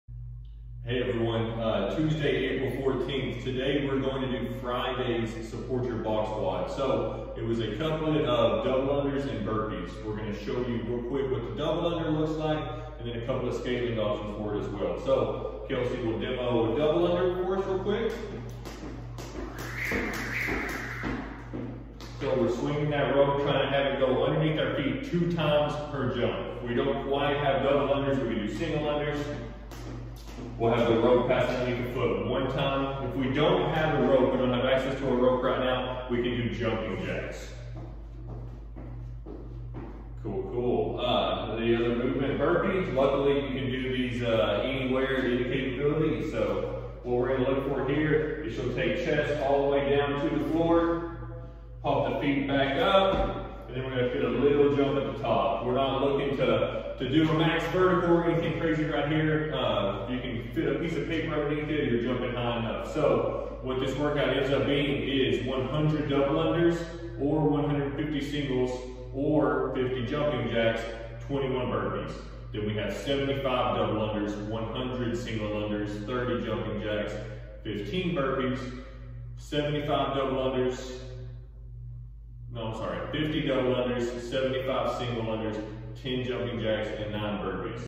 Tuesday 4-14 Discussion
Support Your Box WOD #2
100 Double Unders
21 Burpees
75 Double Unders
15 Burpees
50 Double Unders
9 Burpees
Or,
150 Single Unders/50 Jumping Jacks
21 Burpees
100 Single Unders/30 Jumping Jacks
15 Burpees
75 Single Unders/10 Jumping Jacks
9 Burpees